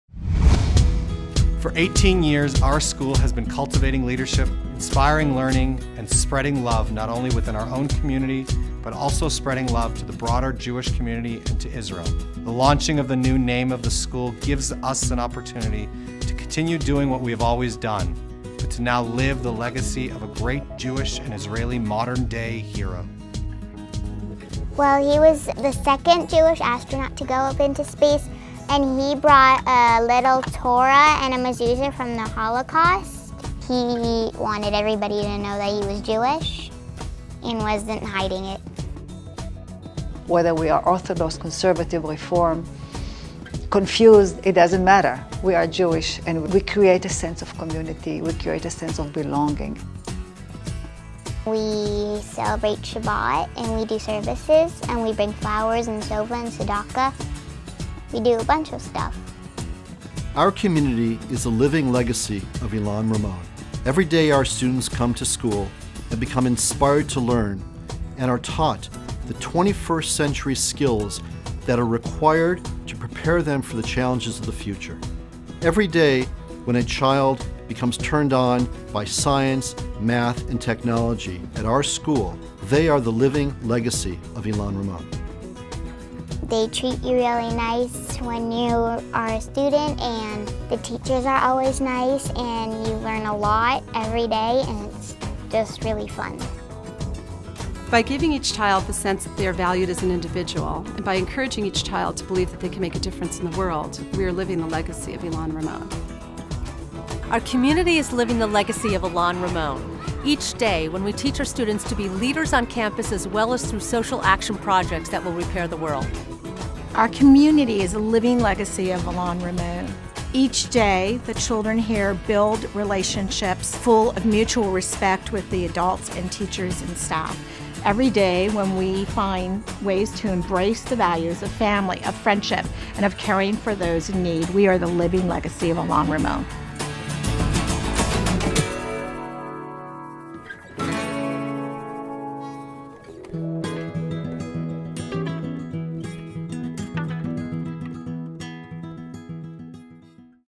Ilan Ramon Day School - Living the Legacy.mov
Ilan Ramon Day School (previously known as Heschel West) is a community Jewish day school that prepares our children to meet the diverse challenges of the 21st Century. Ilan Ramon Day School teaches the whole student with an integrated general and Judaic education that stresses intellectual, spiritual and creative development. We develop committed and enthusiastic lifelong learners and leaders through innovative curriculum and a focus on critical thinking, community involvement and the values of Judaism.